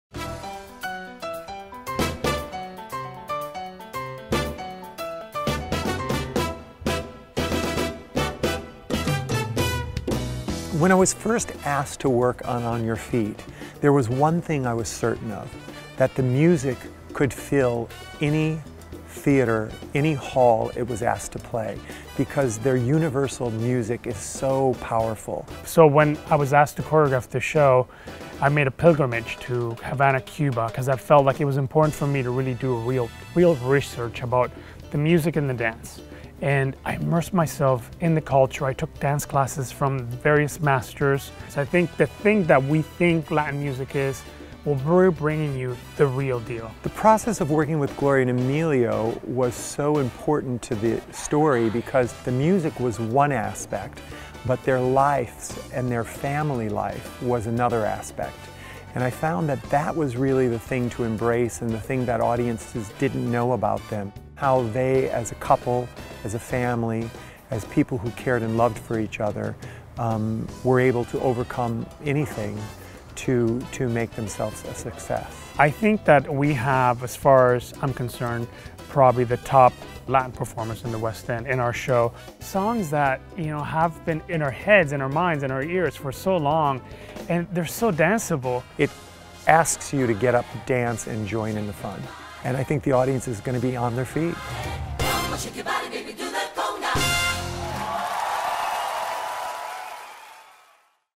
JERRY MITCHELL & SERGIO TRUJILLO
Our wonderful Director Jerry Mitchell & fabulous Choreographer Sergio Trujillo tell us how they brought the story of Gloria Estefan & Emilio Estefan to the stage!